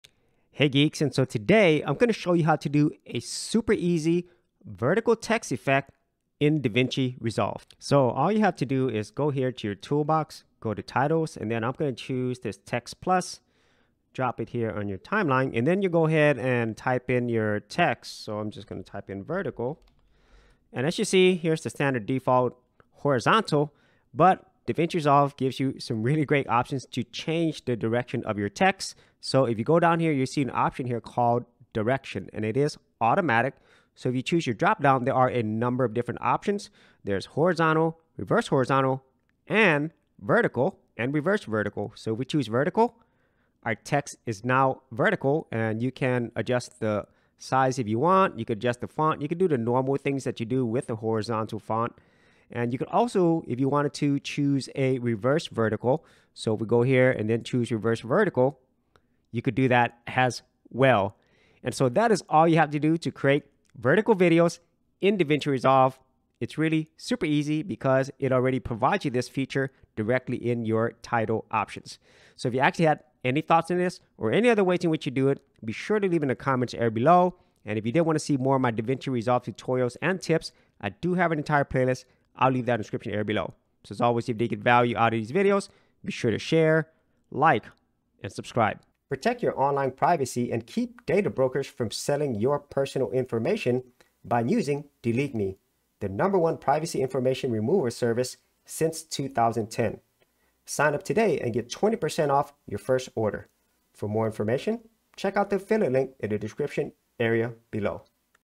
Davinci Resolve Vertical Text (How To Create Vertical Text) | Davinci Resolve Tutorial
Simple Vertical Text For Davinci Resolve!

***Get Social***